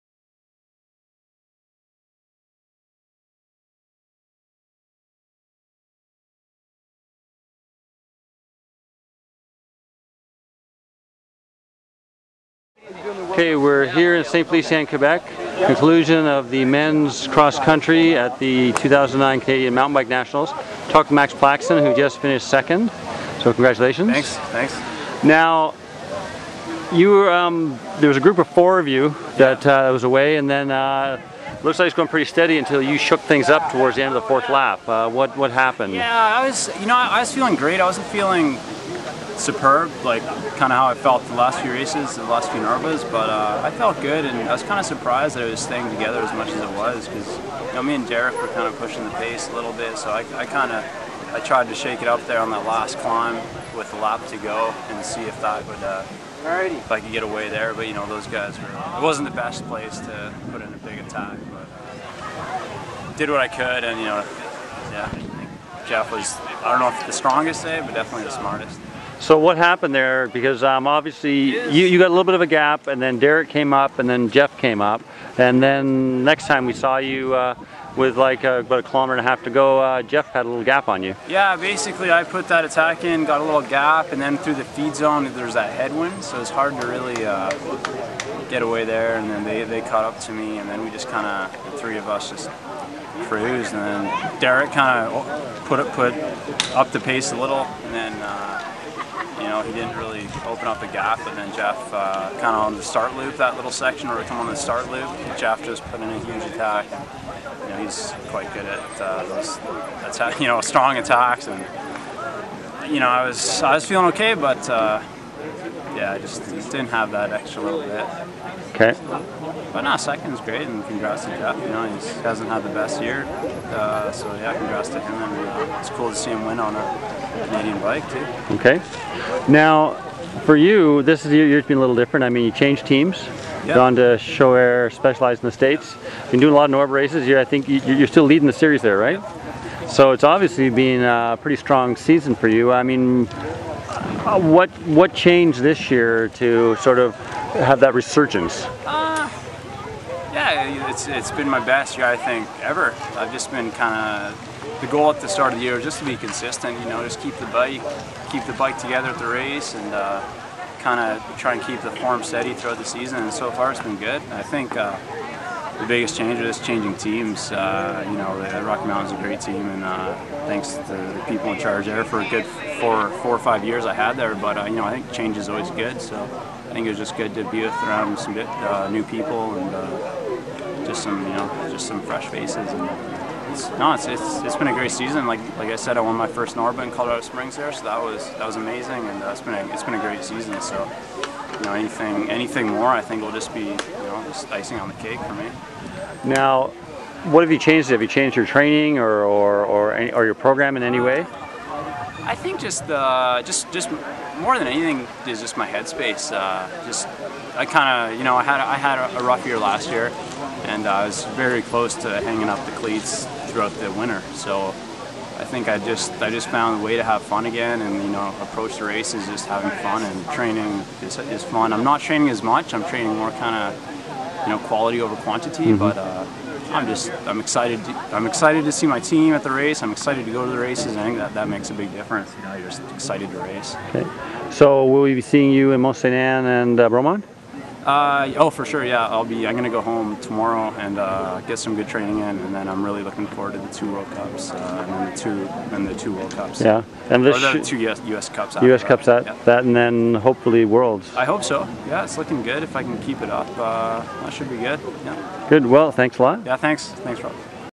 2009 MTB Nationals - Max Plaxton Interview
An interview with Max Plaxton at the 2009 MTB XC Nationals